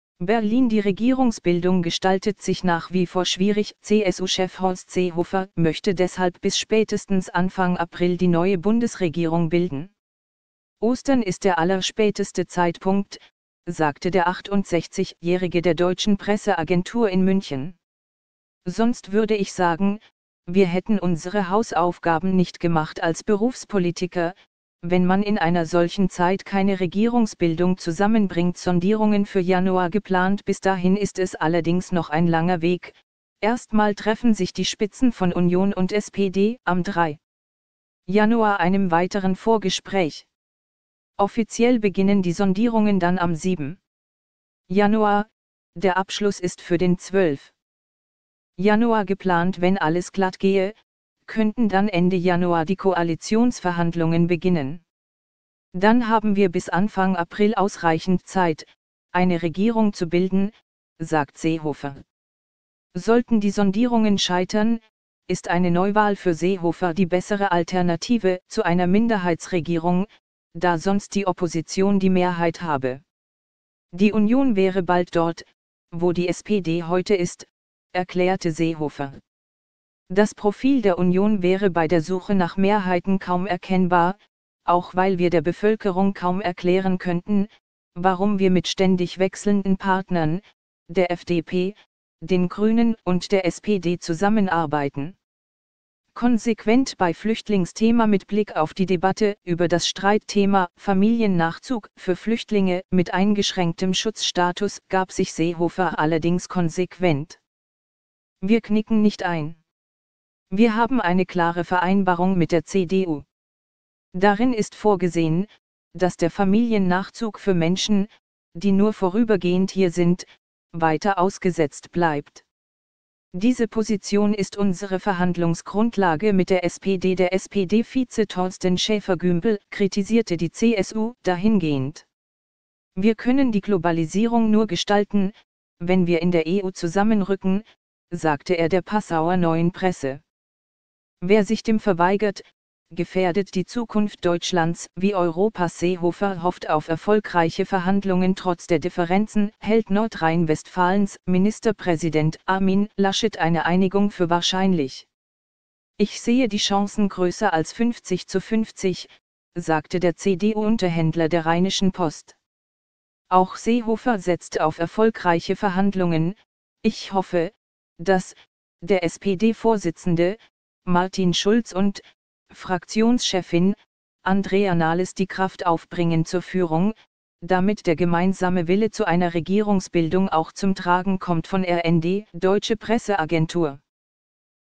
Regierungsbildung – Seehofer fordert GroKo bis Ostern
Regierungsbildung –
Seehofer fordert GroKo bis Ostern